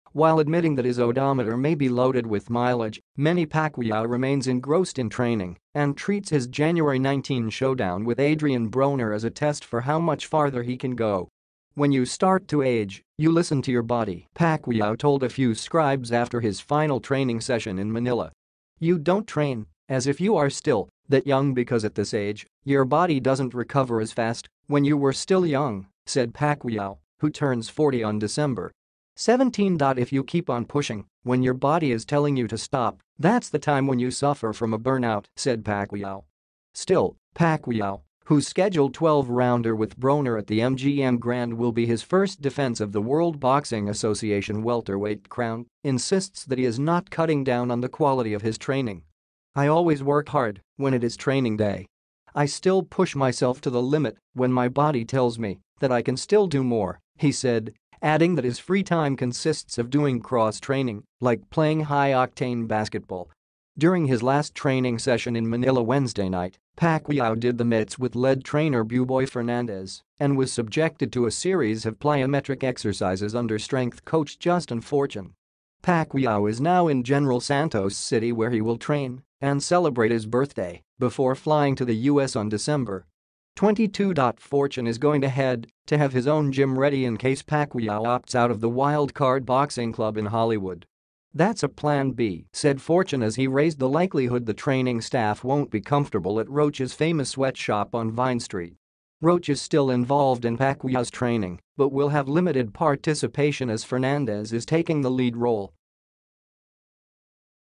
Pacquiao: Listen to what your body says
While admitting that his odometer may be loaded with mileage, Manny Pacquiao remains engrossed in training and treats his January 19 showdown with Adrien Broner as a test for how much farther he can go.

“When you start to age, you listen to your body,” Pacquiao told a few scribes after his final training session in Manila.

“You don’t train as if you are still that young because at this age, your body doesn’t recover as fast (when you were still young),” said Pacquiao, who turns 40 on Dec. 17.

“If you keep on pushing when your body is telling you to stop, that’s the time when you suffer from a burnout,” said Pacquiao.

Still, Pacquiao, whose scheduled 12-rounder with Broner at the MGM Grand will be his first defense of the World Boxing Association welterweight crown, insists that he is not cutting down on the quality of his training.

“I always work hard when it is training day. I still push myself to the limit when my body tells me that I can still do more,” he said, adding that his free time consists of doing cross-training, like playing high-octane basketball.

During his last training session in Manila Wednesday night, Pacquiao did the mitts with lead trainer Buboy Fernandez and was subjected to a series of plyometric exercises under strength coach Justin Fortune.

Pacquiao is now in General Santos City where he will train and celebrate his birthday before flying to the US on Dec. 22.

Fortune is going ahead to have his own gym ready in case Pacquiao opts out of the Wild Card Boxing Club in Hollywood.

“That’s a Plan B,” said Fortune as he raised the likelihood the training staff won’t be comfortable at Roach’s famous sweat shop on Vine Street.

Roach is still involved in Pacquiao’s training but will have limited participation as Fernandez is taking the lead role.